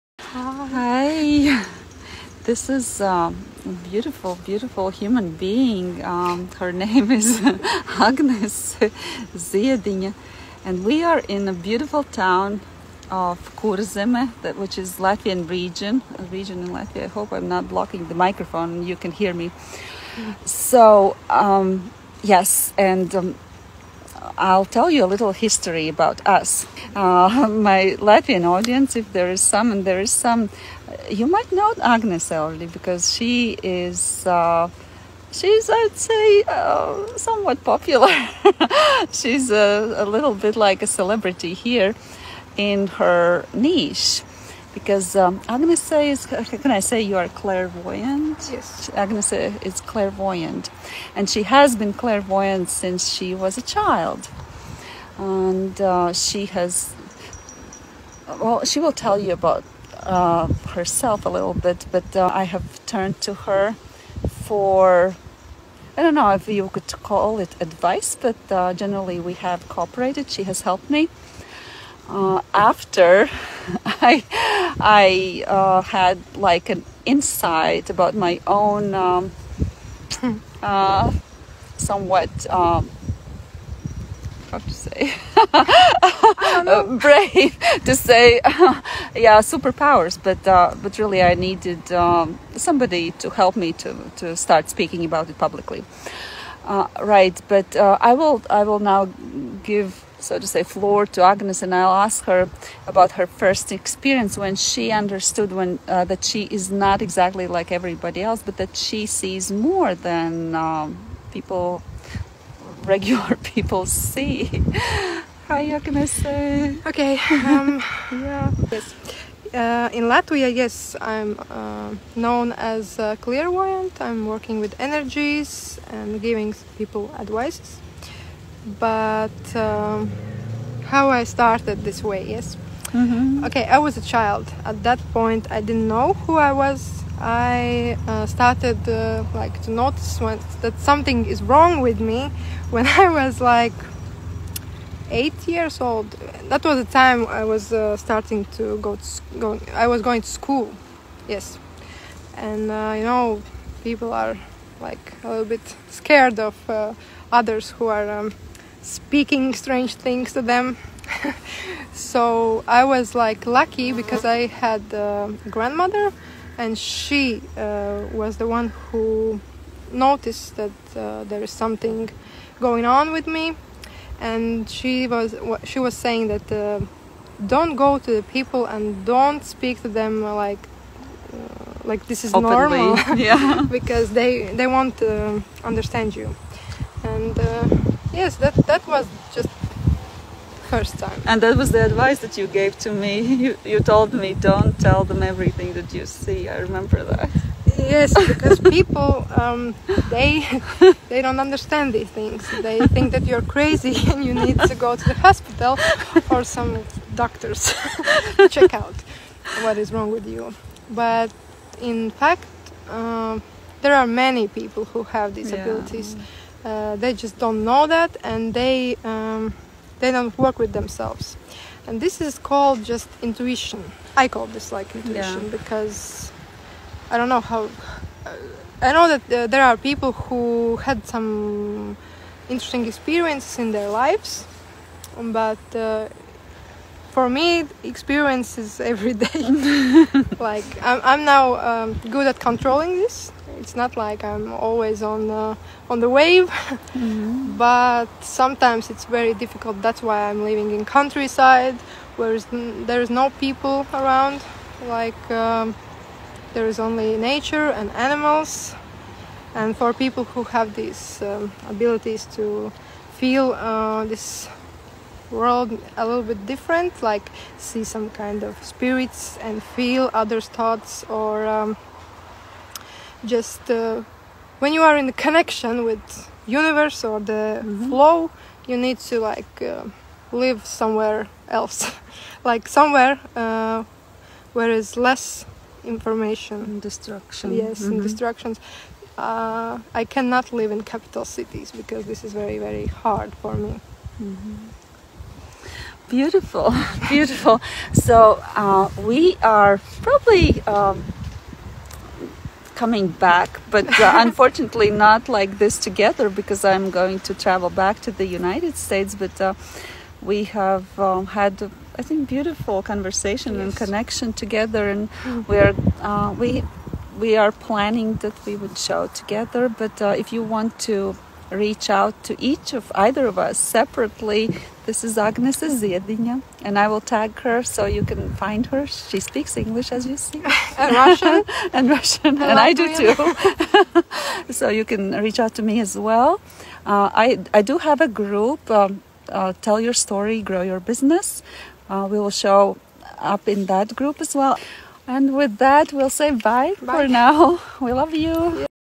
Clairvoyance: Ilze Be meets Agnese Ziediņa in Latvia
This channel features Latvian-born American public speaker, coach, and entrepreneur Ilze Be's professional development journey.